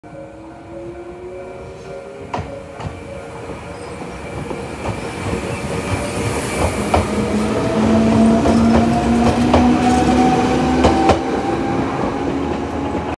【上毛電気鉄道】 天王宿駅 発車シーン
群馬県の上毛電気鉄道 中央前橋行き 天王宿駅発車シーンです。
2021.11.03